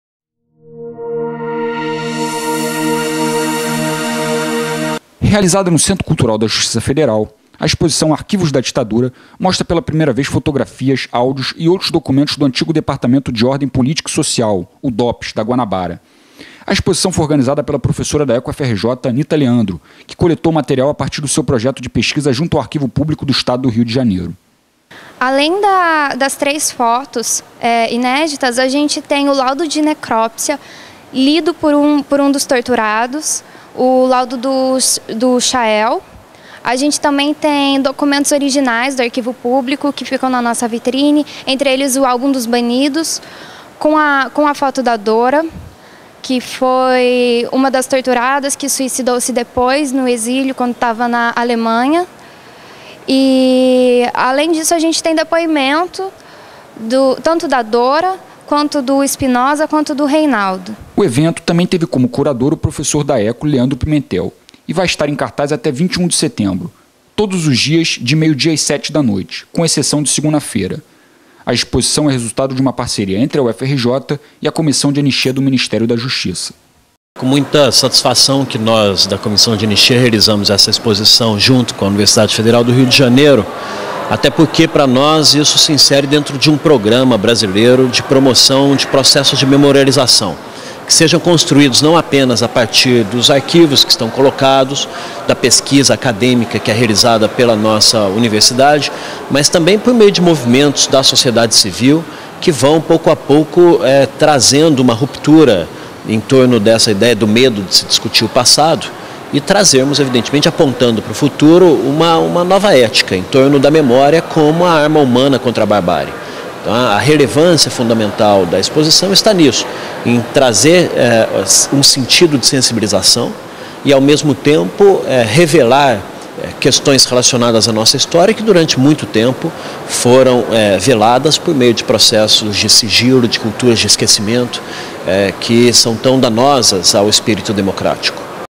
Exposição exibe documentos inéditos da ditadura
Em cartaz no Centro Cultural Justiça Federal (CCJF), "Arquivos da Ditadura" exibe pela primeira vez um conjunto de fotos do antigo Departamento de Ordem Política e Social (DOPS), da Guanabara. Além disso, o evento conta com áudios e outros registros do regime militar.
A exposição foi organizada por Anita Leandro, professora da ECO-UFRJ, que coletou o material ao longo de três anos de pesquisa junto ao Arquivo Público do Estado do Rio de Janeiro. Leandro Pimentel, também professor da ECO, foi o outro curador.
O evento vai até 21 de setembro, todos os dias de 12h às 19h com exceção de segunda-feira, e é fruto de uma parceria entre a UFRJ e o Ministério da Justiça.Em paralelo à exposição, o CCJF receberá entre os dias 12 e 18 de setembro a mostra de filmes também intitulada Arquivos da Ditadura, com 13 produções sobre o período ditatorial realizadas por brasileiros. Entre eles, a estreia nacional do documentário "Retratos de identificação", de Anita Leandro, que foi a grande base para a exposição. E, entre os dias 9 e 11 de setembro, haverá um seminário com pesquisadores  de universidades brasileiras e europeias que vão debater a pesquisa em arquivos.